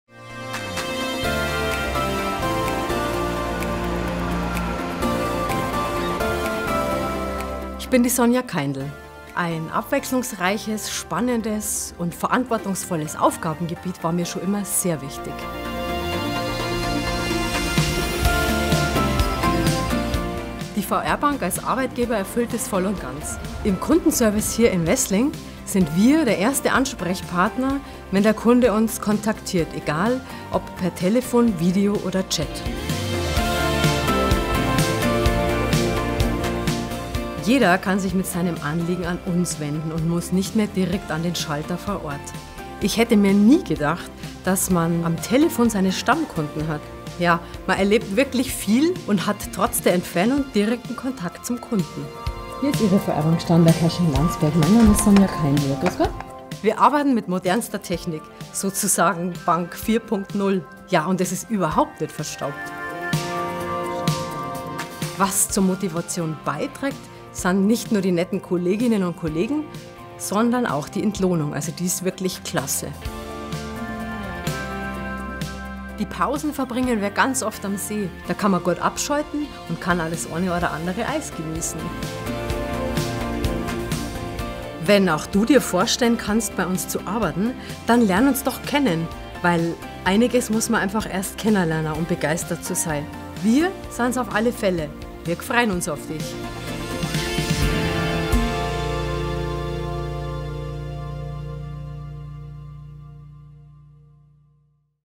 Lust auf eine neue Herausforderung? - Ihr neuer Arbeitsplatz im Kundenservice
Werden Sie Teil eines starken Teams! Der Kundenservice der VR Bank Starnberg-Herrsching-Landsberg eG ist der erste Kontaktpunkt für unsere Kunden. Vielfältige Aufgaben erwarten Sie.